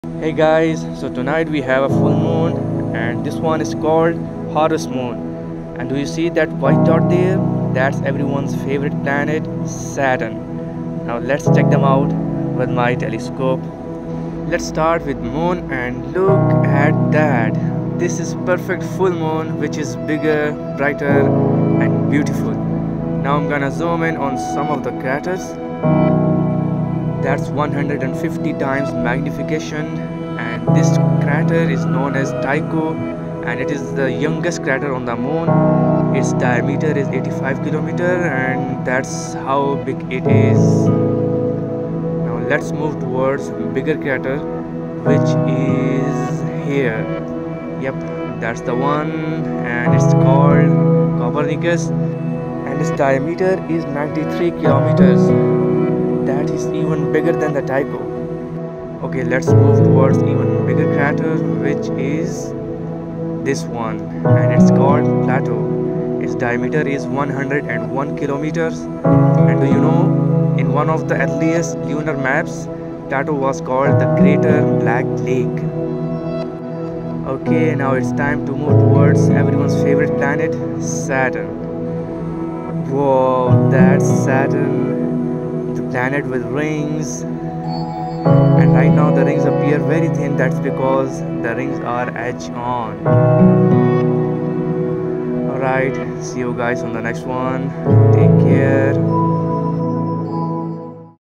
Harvest Moon and Saturn through my telescope 🔭
I captured Harvest Moon and Saturn through my telescope 🔭